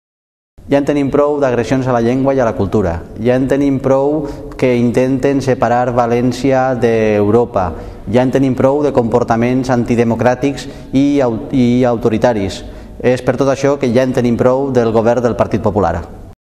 Ja en tenim prou - Toni Gisbert
Una declaració de Toni Gisbert, d'Acció Cultural del País Valencià, per a "Ja en tenim prou"